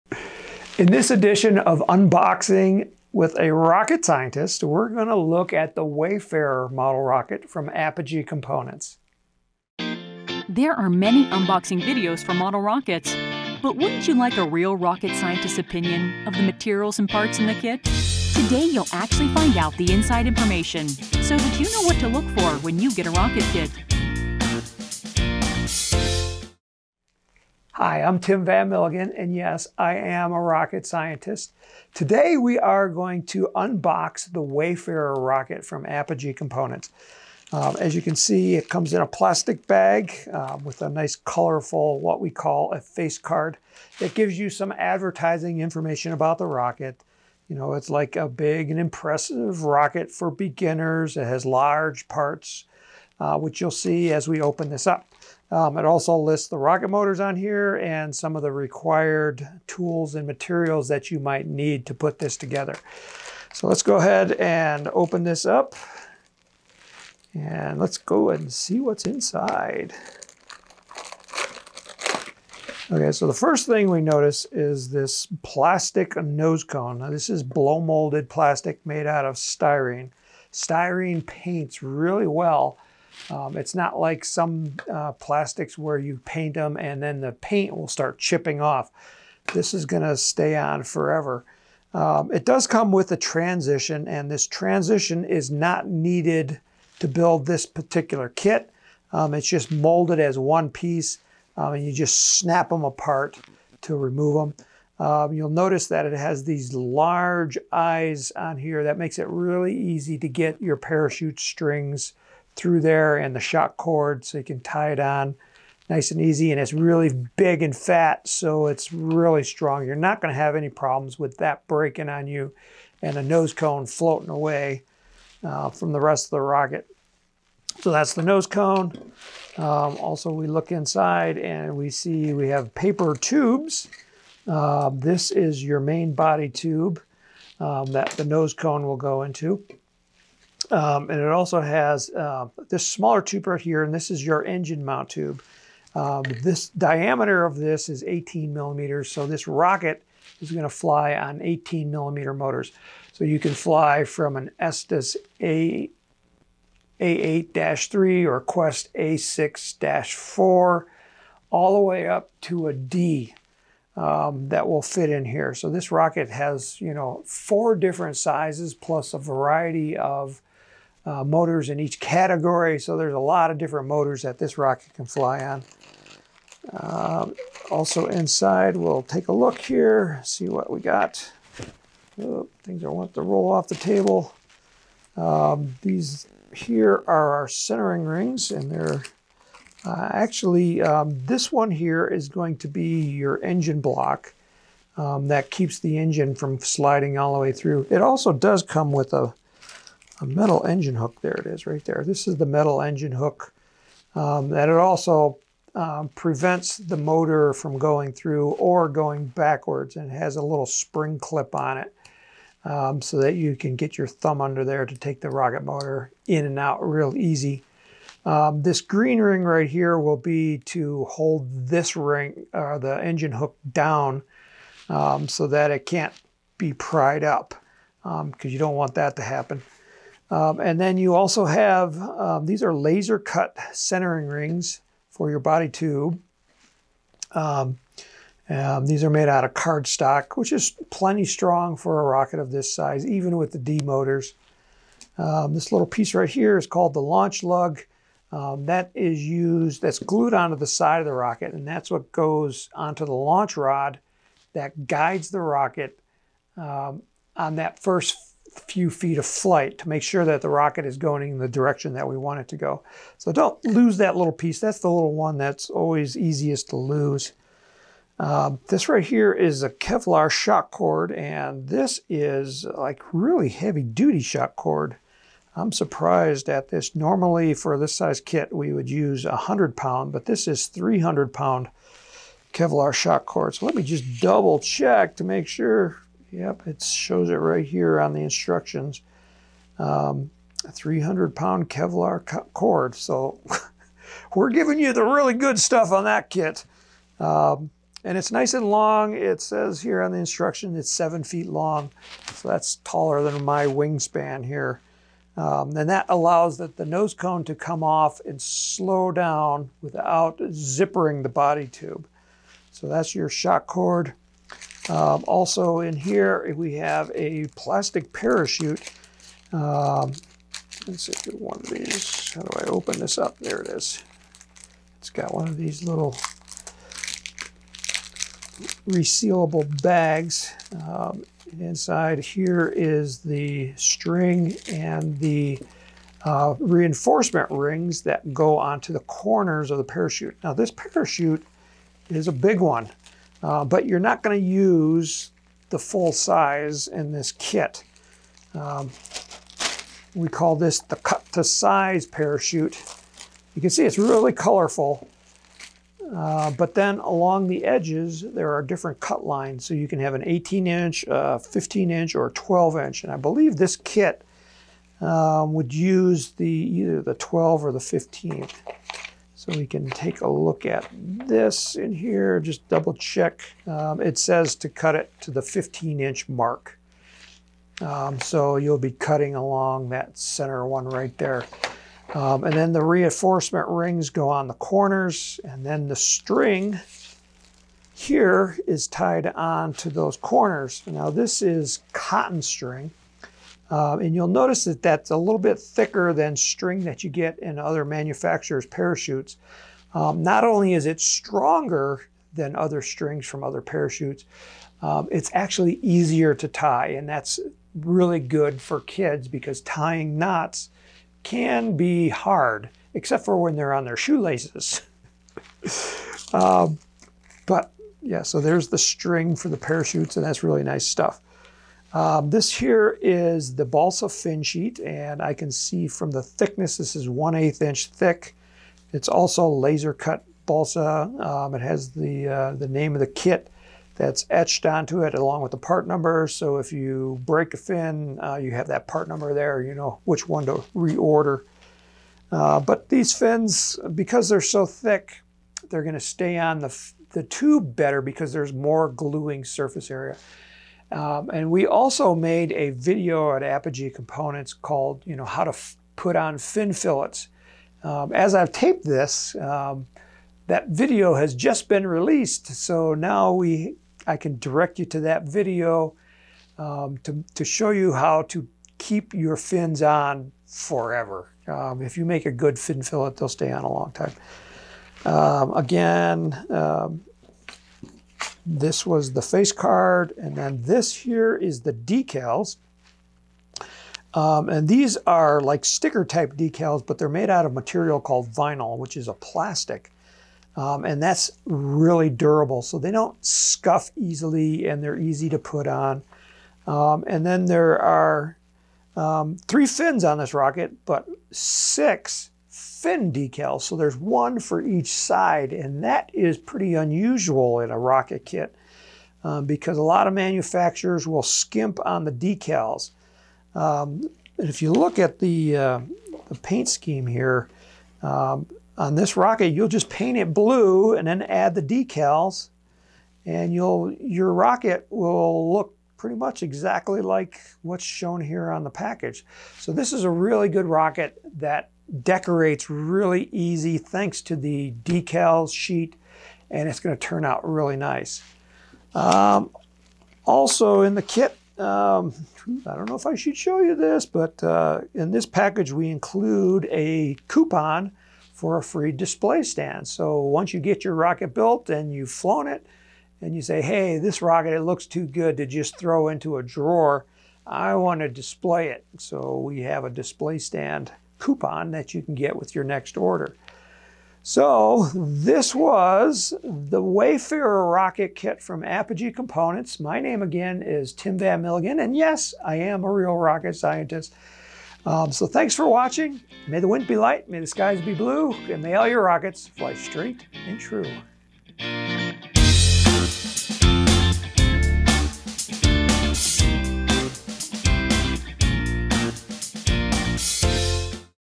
Unboxing the Wayfarer Model Rocket Kit
The Wayfarer is no small model rocket. It is a full 15 inches tall making it one of the largest truly beginner appropriate models available. Between its size and the bold graphics, this is a rocket with some serious personality. This is an excellent kit for the beginner who wants something just a bit larger than some of our other entry-level rockets without building becoming more expensive, the field needing to be larger, or the skills required more advanced. We've also found that it makes an excellent base for new designs with its slightly stubby looks and jet-like fins.